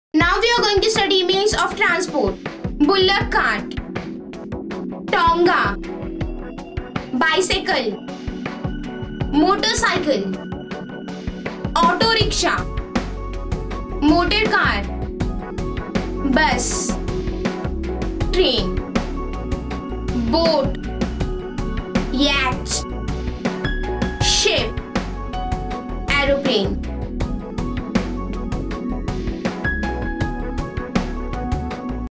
TRANSPORT
Name of Transport
Different means of Transport Learning for Kids
dear, Student welcome to CWT Educational channel.
By: Afin B Alam Pathan

educational activities for kids
english rhymes lyrics
english songs for kids
nursery rhymes on transport
transport for kids learning